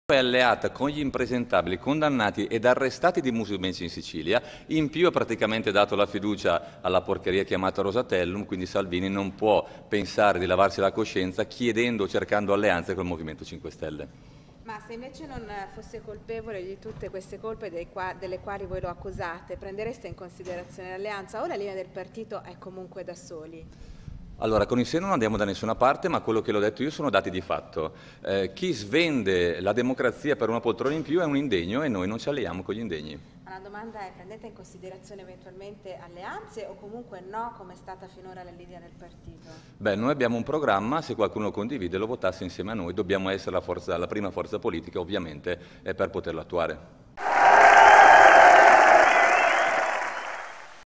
Toninelli (Movimento 5 Stelle) contro la Lega: "Indegna, non ci alleiamo"
Duro attacco alla Lega di Matteo Salvini da parte del deputato del M5S Danilo Toninelli. “La Lega è come tutti gli altri - ha detto in un'intervista alla Camera - in Sicilia sta con l’impresentabile Musumeci e una settimana fa ha addirittura votato la fiducia al Rosatellum svendendo così la democrazia: è indegna e Salvini e noi non ci alleiamo con gli indegni". Lo ha detto il deputato dei 5 stelle Danilo Toninelli, parlando con i cronisti alla Camera. A chi gli chiedeva se il Movimento 5 Stelle consideri o rifiuti a priori alleanze con altri partiti, Toninelli ha risposto: “ Noi abbiamo un programma e se qualcuno è d'accordo lo voti insieme a noi;è chiaro che dobbiamo essere la prima forza politica per poterlo attuare".